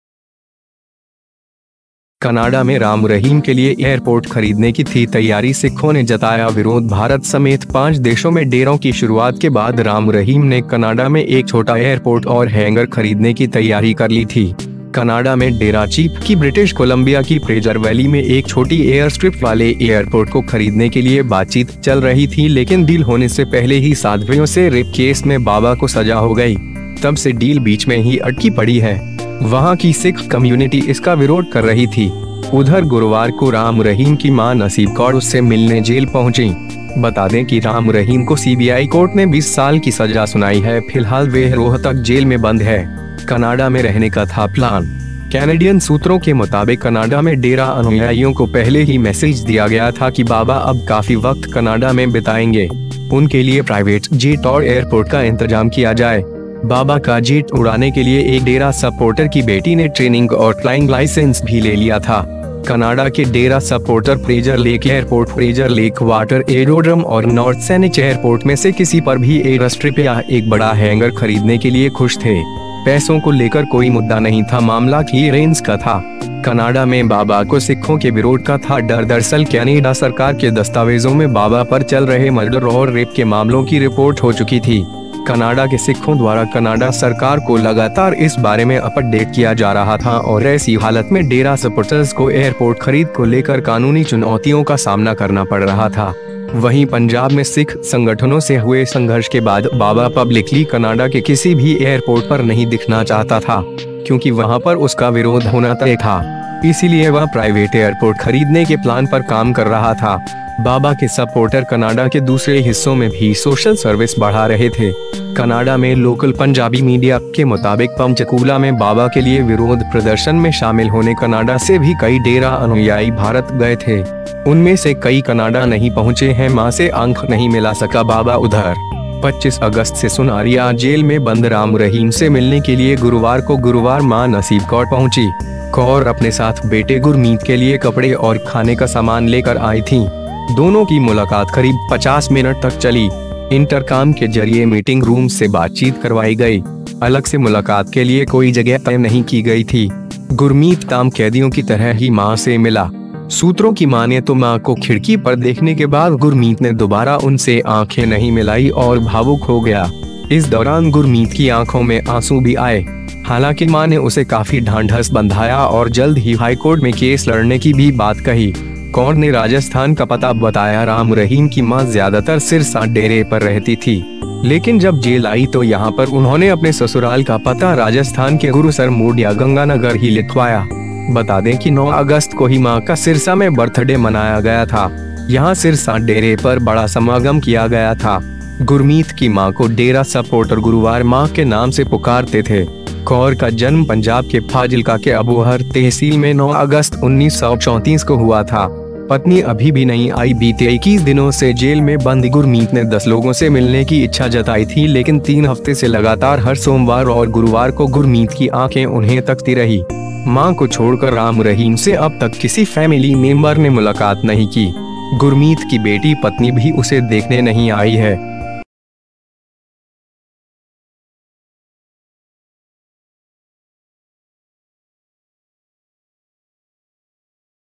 कनाडा में राम रहीम के लिए एयरपोर्ट खरीदने की थी तैयारी, सिखों ने जताया विरोध
भारत समेत 5 देशों में डेरों की शुरुआत के बाद राम रहीम ने कनाडा में एक छोटा एयरपोर्ट और हैंगर खरीदने की तैयारी कर ली थी। कनाडा में डेरा चीफ की ब्रिटिश कोलंबिया की फ्रेजर वैली में एक छोटी एयर स्ट्रिप वाले एयरपोर्ट को खरीदने के लिए बातचीत चल रही थी, लेकिन डील होने से पहले ही साध्वियों से रेप केस में बाबा को सजा हो गई। तब से डील बीच में ही अटकी पड़ी है। वहां की सिख कम्युनिटी इसका विरोध कर रही थी। उधर, गुरुवार को राम रहीम की मां नसीब कौर उससे मिलने जेल पहुंचीं। बता दें कि राम रहीम को सीबीआई कोर्ट ने 20 साल की सजा सुनाई है, फिलहाल वह रोहतक जेल में बंद है। कनाडा में रहने का था प्लान...
- कैनेडियन सूत्रों के मुताबिक, कनाडा में डेरा अनुयायियों को पहले ही मैसेज दिया गया था कि बाबा अब काफी वक्त कनाडा में बिताएंगे। उनके लिए प्राइवेट जेट और एयरपोर्ट का इंतजाम किया जाए।
- बाबा का जेट उड़ाने के लिए एक डेरा सपोर्टर की बेटी ने ट्रेनिंग और फ्लाइंग लाइसेंस भी ले लिया था। कनाडा के डेरा सपोर्टर फ्रेजर लेक एयरपोर्ट, फ्रेजर लेक वाटर एयरोड्रम और नॉर्थ सैनिच एयरपोर्ट में से किसी पर भी एयरस्ट्रिप या एक बड़ा हैंगर खरीदने के लिए खुश थे। पैसों को लेकर कोई मुद्दा नहीं था, मामला क्लीयरेंस का था।
कनाडा में बाबा को सिखों के विरोध का था डर
- दरअसल, कैनेडा सरकार के दस्तावेजों में बाबा पर चल रहे मर्डर और रेप के मामलों की रिपोर्ट हो चुकी थी। कनाडा के सिखों द्वारा कनाडा सरकार को लगातार इस बारे में अपडेट किया जा रहा था और ऐसी हालत में डेरा सपोटर्स को एयरपोर्ट खरीद को लेकर कानूनी चुनौतियों का सामना करना पड़ रहा था।
- वहीं पंजाब में सिख संगठनों से हुए संघर्ष के बाद बाबा पब्लिकली कनाडा के किसी भी एयरपोर्ट पर नहीं दिखना चाहता था, क्योंकि वहां पर उसका विरोध होना तय था। इसीलिए वह प्राइवेट एयरपोर्ट खरीदने के प्लान पर काम कर रहा था। बाबा के सपोर्टर कनाडा के दूसरे हिस्सों में भी सोशल सर्विस बढ़ा रहे थे।
- कनाडा में लोकल पंजाबी मीडिया के मुताबिक, पंचकूला में बाबा के लिए विरोध प्रदर्शन में शामिल होने कनाडा से भी कई डेरा अनुयायी भारत गए थे। उनमें से कई कनाडा नहीं पहुंचे हैं।मां से आंख नहीं मिला सका बाबा
- उधर, 25 अगस्त से सुनारिया जेल में बंद राम रहीम से मिलने के लिए गुरुवार को गुरु मां नसीब कौर पहुंची। कौर अपने साथ बेटे गुरमीत के लिए कपड़े और खाने का सामान लेकर आई थीं।
- दोनों की मुलाकात करीब 50 मिनट तक चली। इंटरकाम के जरिए मीटिंग रूम से बातचीत करवाई गई। अलग से मुलाकात के लिए कोई जगह तय नहीं की गई थी। गुरमीत आम कैदियों की तरह ही मां से मिला।
- सूत्रों की मानें तो मां को खिड़की पर देखने के बाद गुरमीत ने दोबारा उनसे आंखें नहीं मिलाई और भावुक हो गया। इस दौरान गुरमीत की आंखों में आंसू भी आए। हालांकि मां ने उसे काफी ढांढस बंधाया और जल्द ही हाईकोर्ट में केस लड़ने की भी बात कही।
कौर ने राजस्थान का पता बताया
- राम रहीम की मां ज्यादातर सिरसा डेरे पर रहती थी, लेकिन जब जेल आई तो यहां पर उन्होंने अपने ससुराल का पता राजस्थान के गुरुसर मोडिया गंगानगर ही लिखवाया। बता दें कि 9 अगस्त को ही मां का सिरसा में बर्थडे मनाया गया था। यहां सिरसा डेरे पर बड़ा समागम किया गया था।
- गुरमीत की मां को डेरा सपोर्टर गुरु मां के नाम से पुकारते थे। कौर का जन्म पंजाब के फाजिल्का के अबोहर तहसील में 9 अगस्त 1934 को हुआ था।
पत्नी अभी भी नहीं आई
- बीते 21 दिनों से जेल में बंद गुरमीत ने 10 लोगों से मिलने की इच्छा जताई थी, लेकिन तीन हफ्ते से लगातार हर सोमवार और गुरुवार को गुरमीत की आंखें उन्हें तकती रही।
- मां को छोड़कर राम रहीम से अब तक किसी फैमिली मेंबर ने मुलाकात नहीं की। गुरमीत की बेटी-पत्नी भी उसे देखने नहीं आई है।